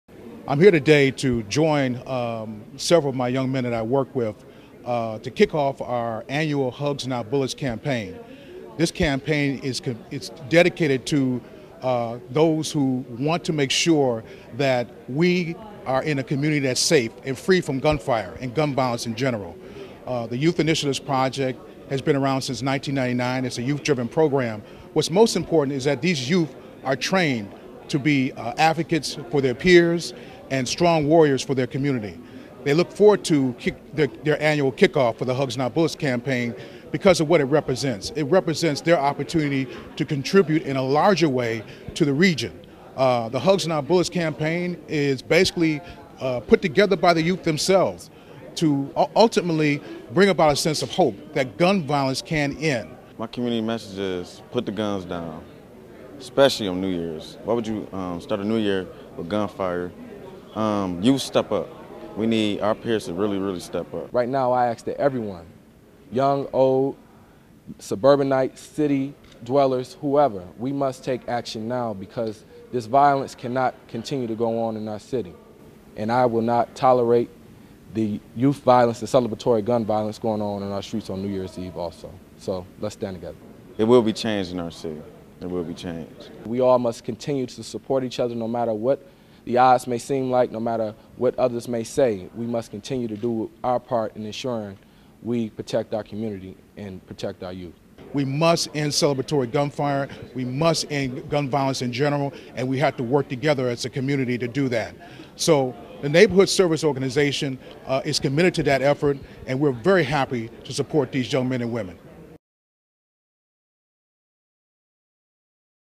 Hugs, Not Bullets
Local student activists from the Detroit Youth Initiatives Project announce a New Year's Eve campaign that promotes "Hugs, Not Bullets" as a cease-fire alternative to the practice of shooting guns into the air at midnight.